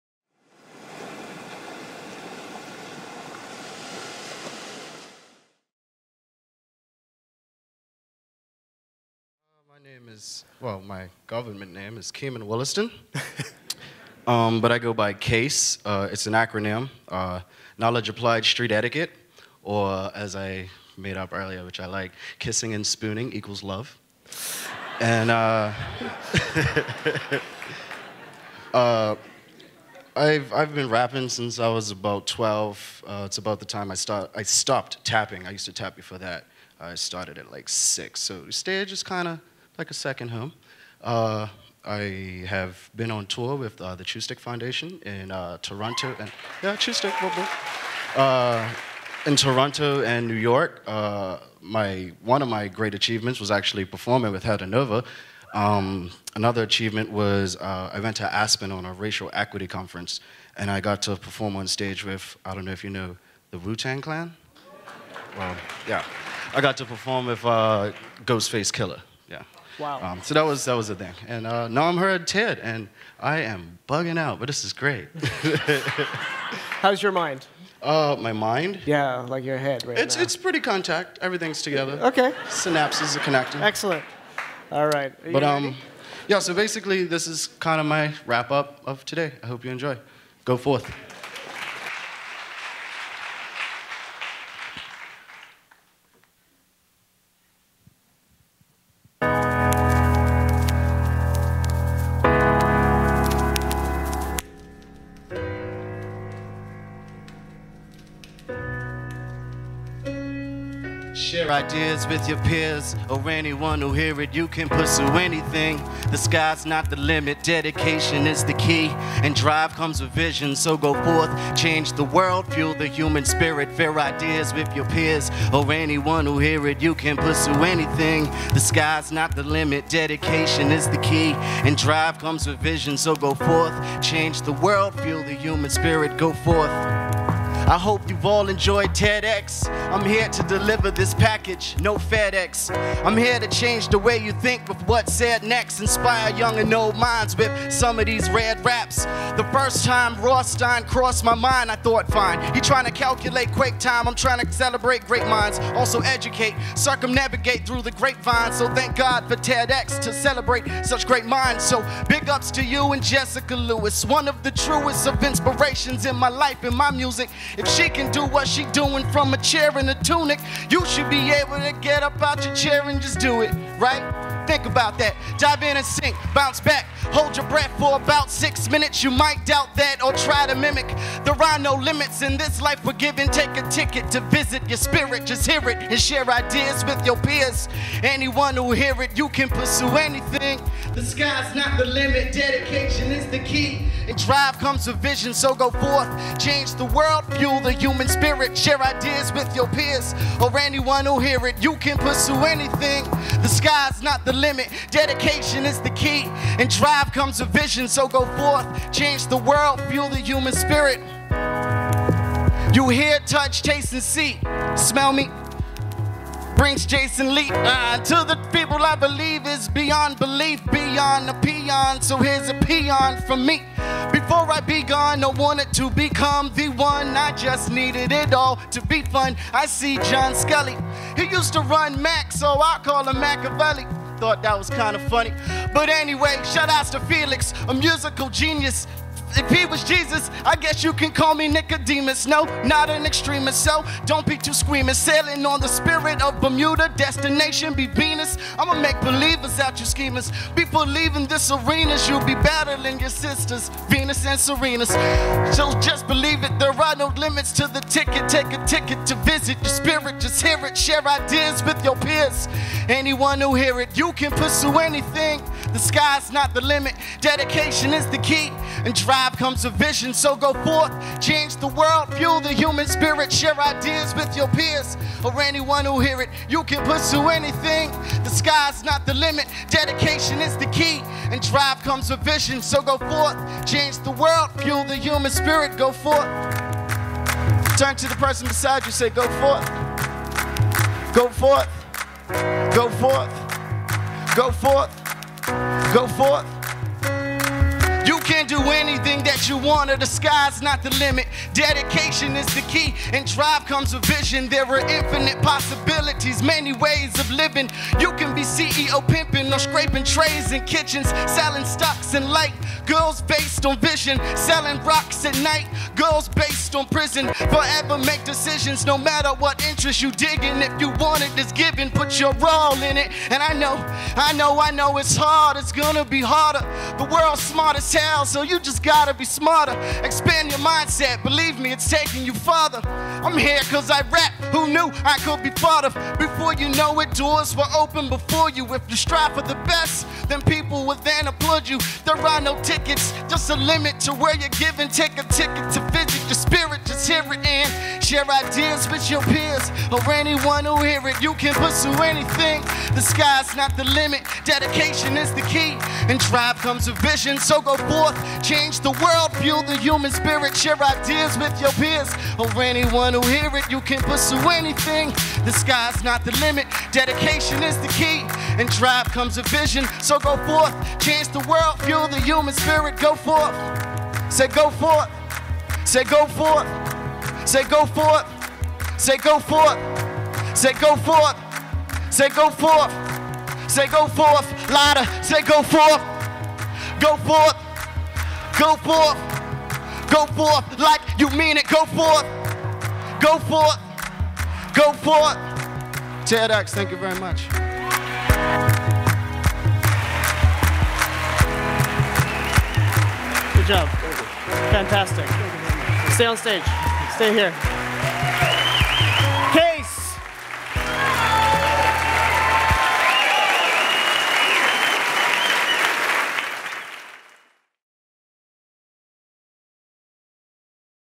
It's a (W)rap: KASE at TEDxBermuda
KASE began rapping at 15, a rhythmic progression from childhood tap dancing. His infectious rhymes are regularly heard on Bermuda radio and he is a staple on the local Bermuda music circuit.